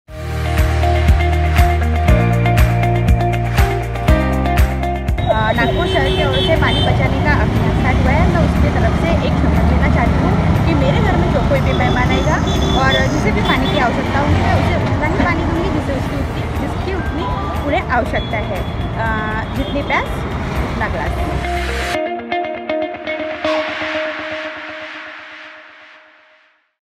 भविष्य को सुरक्षित बनाना है तो जल बचाए. 'जितनी प्यास, उतना ग्लास'
'नवभारत-नवराष्ट्र' व ओसीडब्ल्यू द्वारा चलाये जा रहे जल-कल अभियान के तहत शहर के चौराहों पर 'हाफ ग्लास पानी' उपक्रम 'जितनी प्यास, उतना पानी' की शुरुआत की गई है. इस उपक्रम के माध्यम से शहर के सभी होटल व रेस्टारेंट सहित अन्य से अपील की गई है कि आधा ग्लास ही सर्व करें.